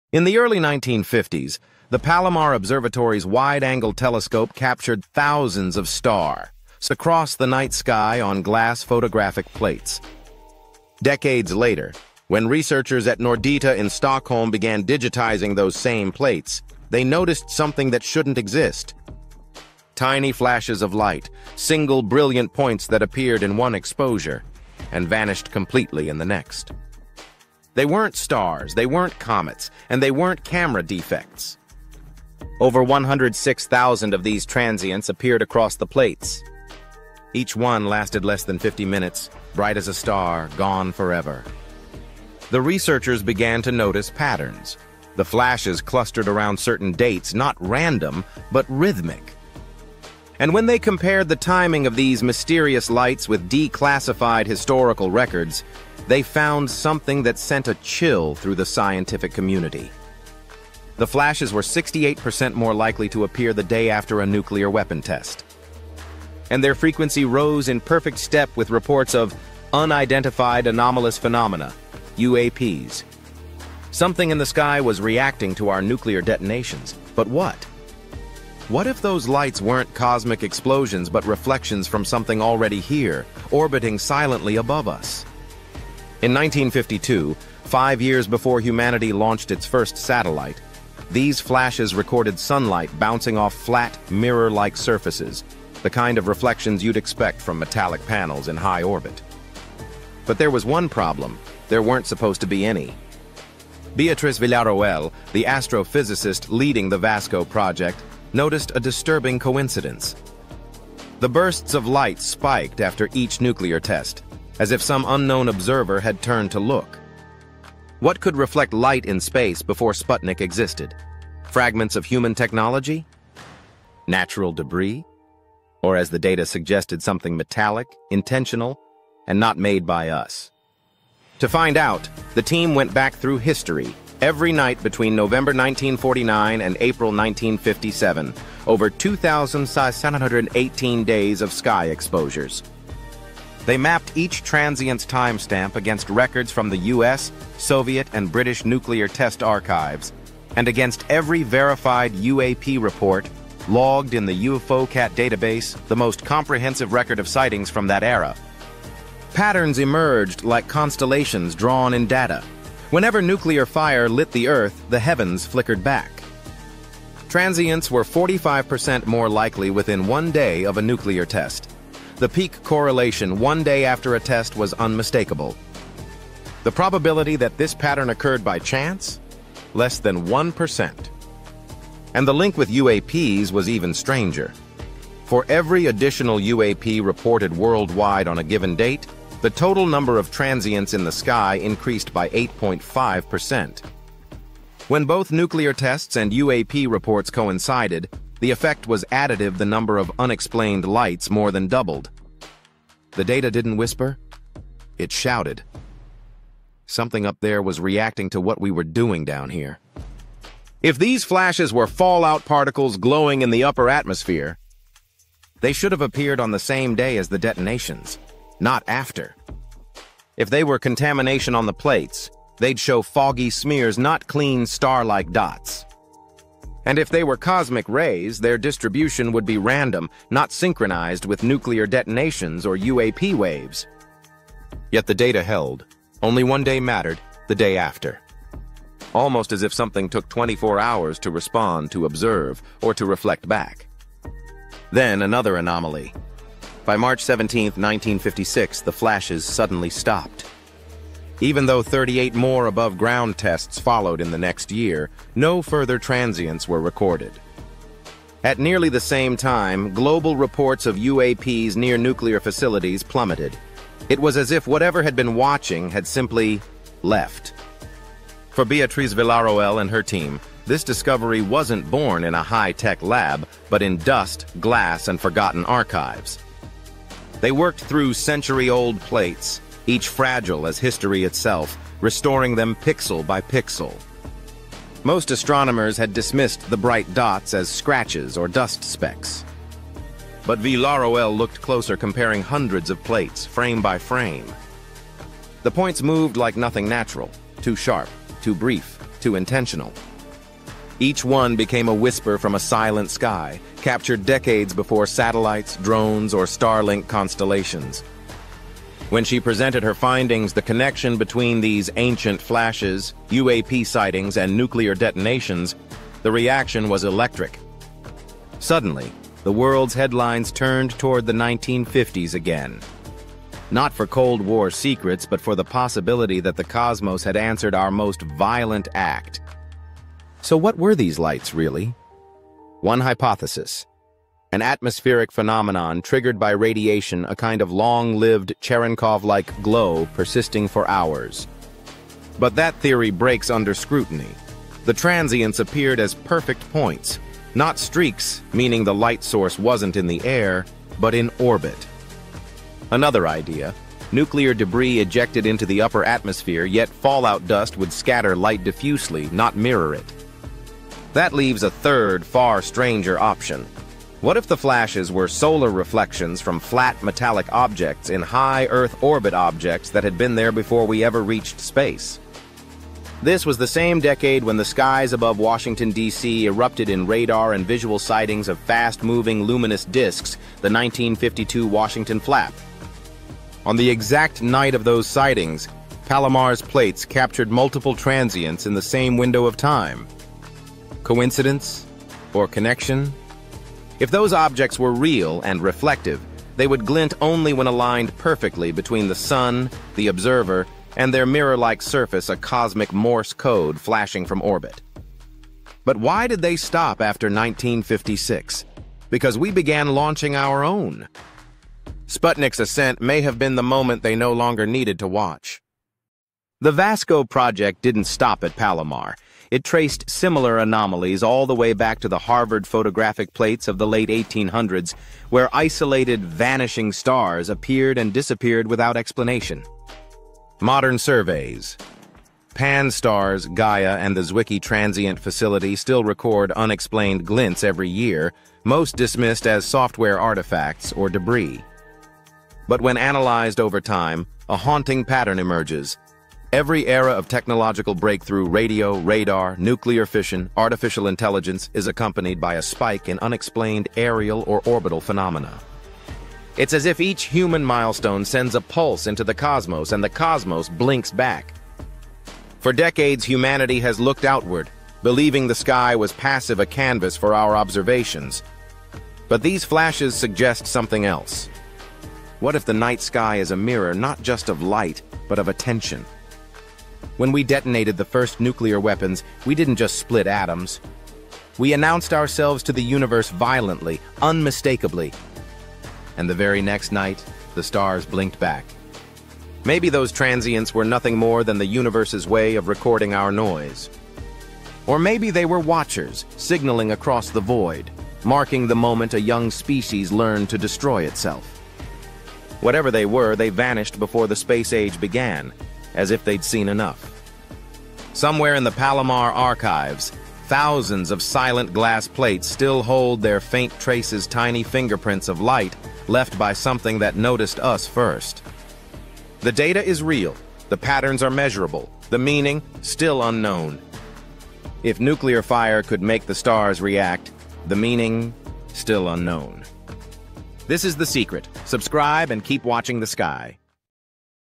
Nuclear Test And UAPs Association: The Hidden Connection
In the shadowy archives of the 1950s, astronomers at Palomar Observatory captured the night sky on fragile glass plates, unknowingly recording over 106,000 fleeting flashes of light—mysterious transients that appeared and vanished in under 50 minutes. These weren't stars, comets, or mere defects; they clustered rhythmically, spiking by 68% the day after nuclear weapon tests. Led by astrophysicist Beatriz Villarroel, the VASCO project unearthed chilling correlations: these celestial anomalies aligned with declassified nuclear detonation records and global UAP (UFO) sightings, increasing by 45% post-blast and an additional 8.5% per reported UAP. With odds of random chance below 1%, the data suggests these flashes could be solar reflections from metallic, mirror-like objects in high orbit—predating humanity's first satellite, Sputnik, by years. Were they natural debris, atmospheric fallout, or evidence of extraterrestrial observers reacting to our atomic experiments? As nuclear fires lit the Earth, the heavens seemed to blink back, only to fall silent after 1956 despite ongoing tests, mirroring a drop in UAP reports near nuclear sites. Delve into this cosmic enigma, where human milestones in technology echo through the stars, hinting at a watchful universe.